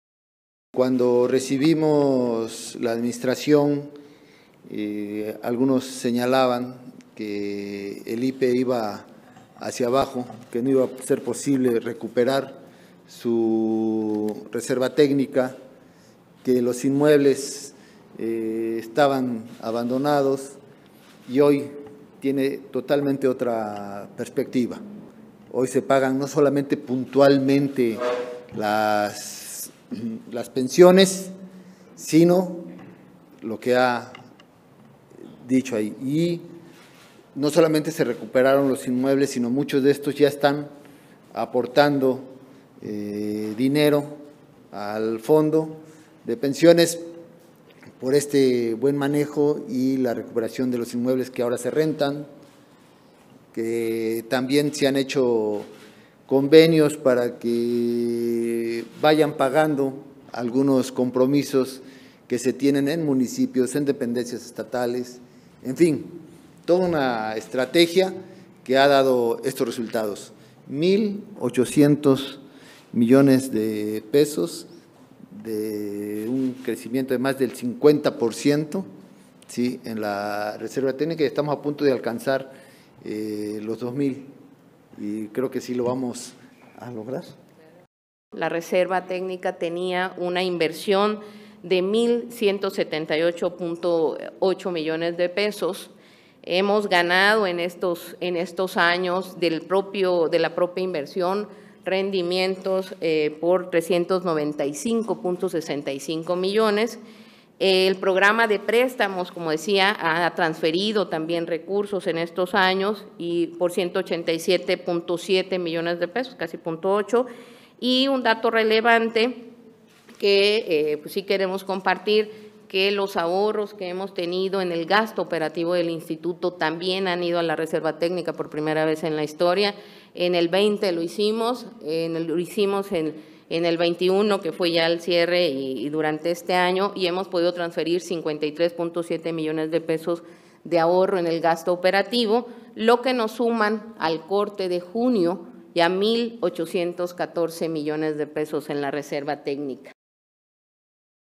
Para 2023, el IPE prevé alcanzar 2 mil mdp en su Reserva Técnica: Gobernador Cuitláhuac
Video de 46 9039